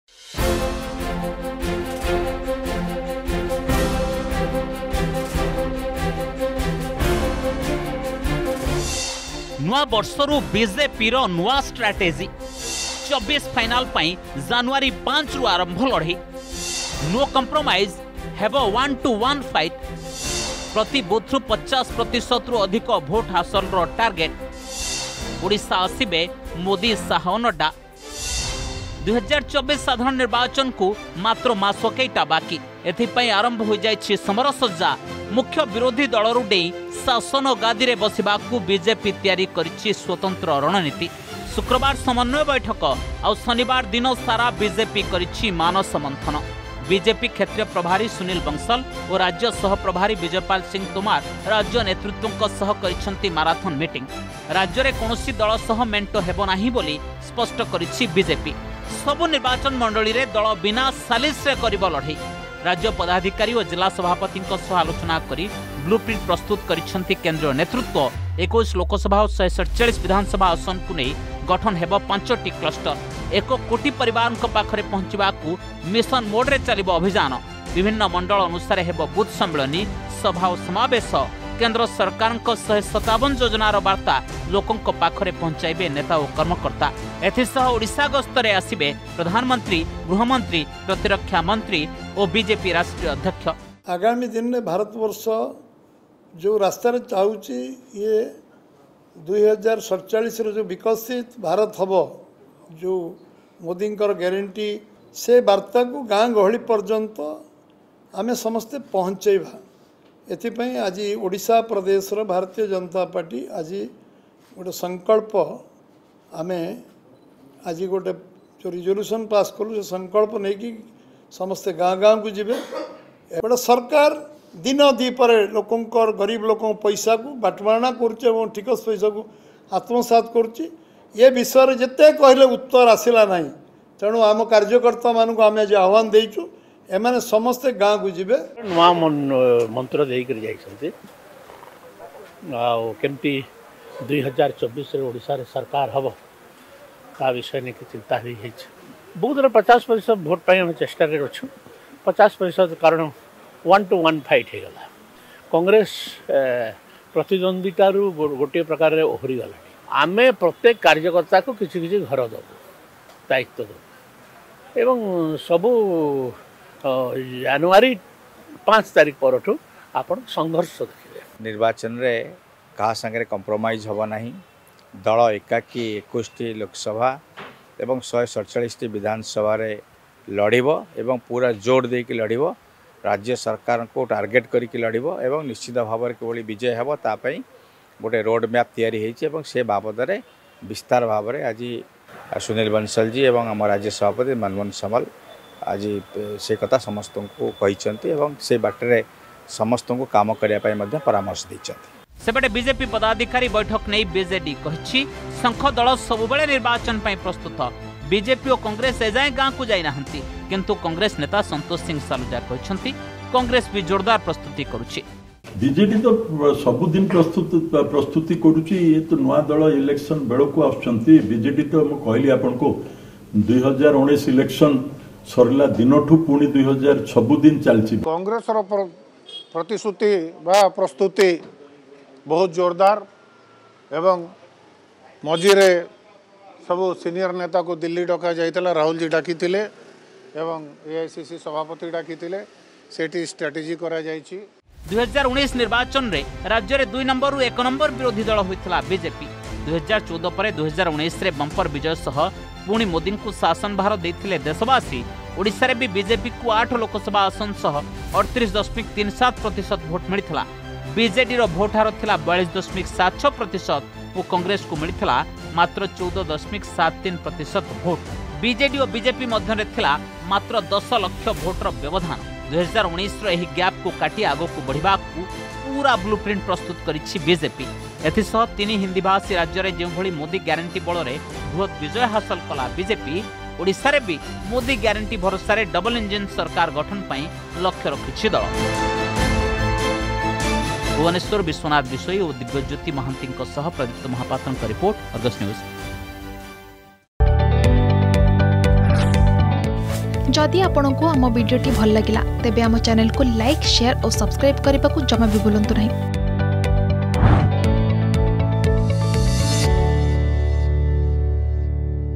ନୂଆବର୍ଷରୁ ବିଜେପିର ନୂଆ ଷ୍ଟ୍ରାଟେଜି || Election 2024 || BJP || Odisha
ନୂଆବର୍ଷରୁ ବିଜେପିର ନୂଆ ଷ୍ଟ୍ରାଟେଜି । 24 ଫାଇନାଲ୍ ପାଇଁ ଜାନୁଆରୀ 5ରୁ ଆରମ୍ଭ ହେବ ଲଢ଼େଇ । ପଦାଧିକାରୀ ଓ ଜିଲ୍ଲା ସଭାପତି ବୈଠକରେ ନିଆଯାଇଛି ବଡ ନିଷ୍ପତ୍ତି ।